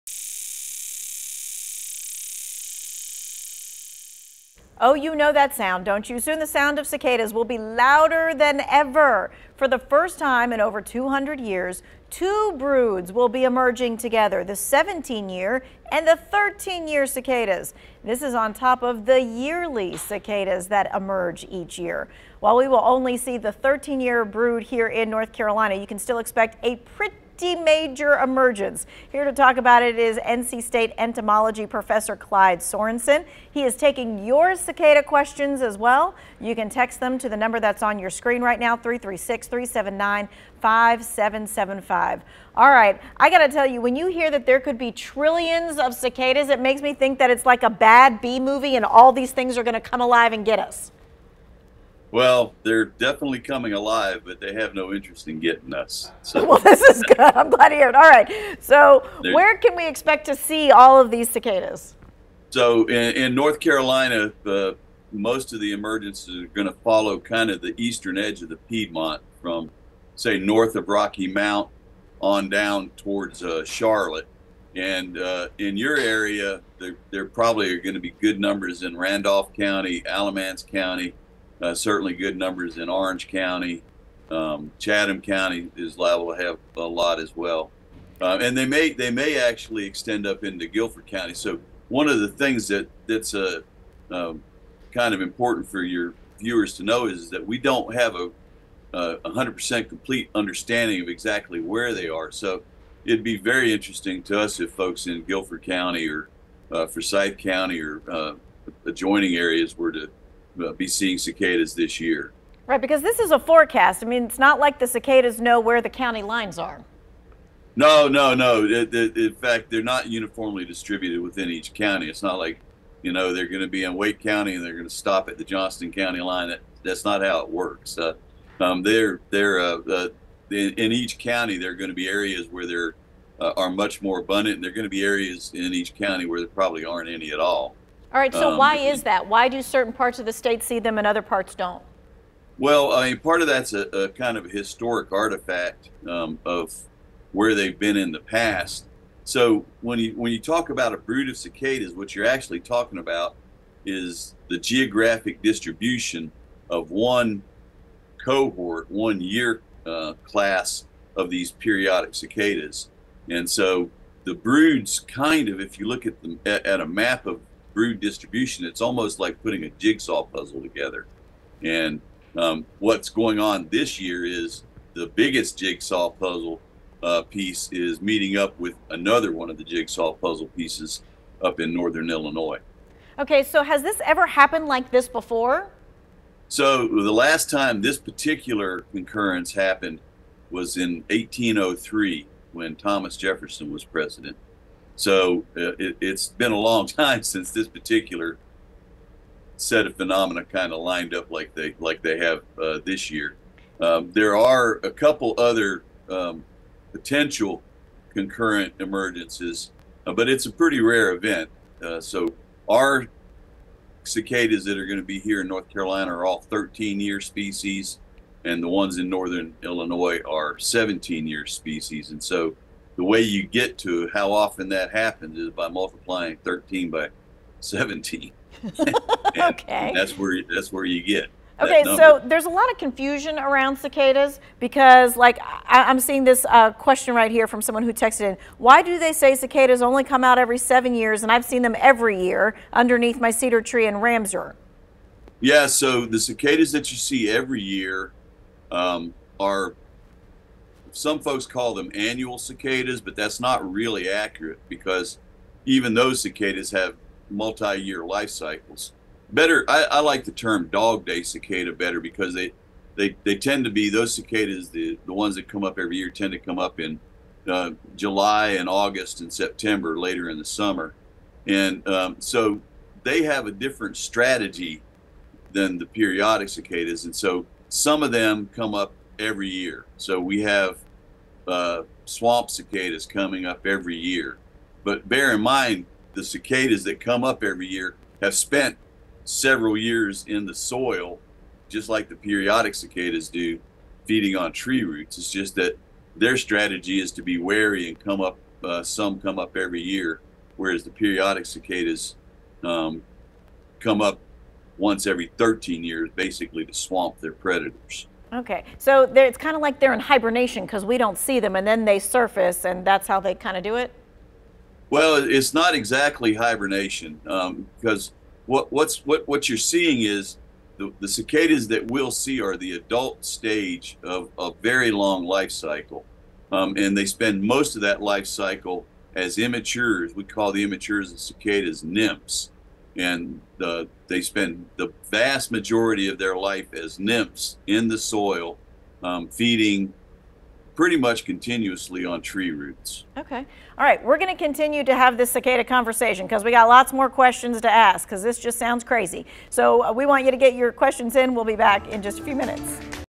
Are cicadas harmful to humans?
NC State professor answers your questions about the historic cicada emergence happening this year.